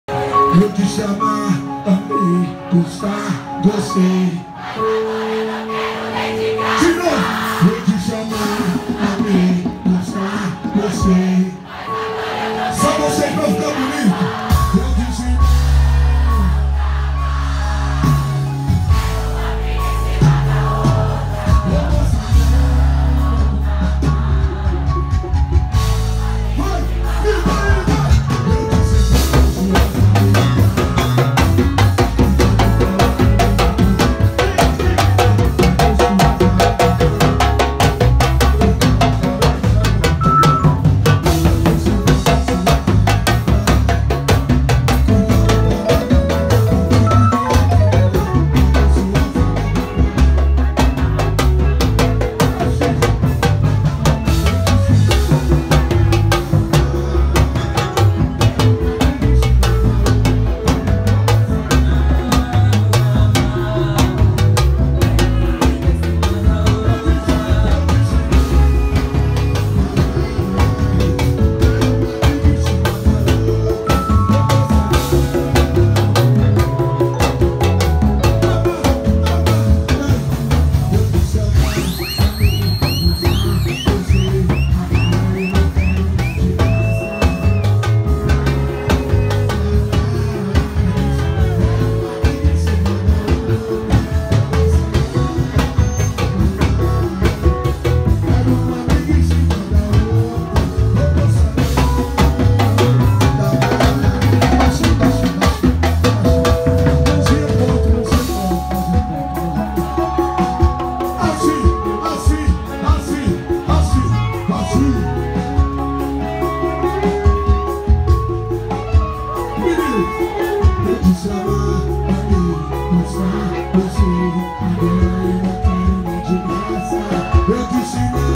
Bloco do Fervo - MegaBloco do Carnaval de Rua de São Paulo - Fervo Samba Show
O Maior Bloco do Carnaval de Rua da Zona Norte de São Paulo.
Bloco do Fervo
Edição 2019
Público: 80 mil pessoas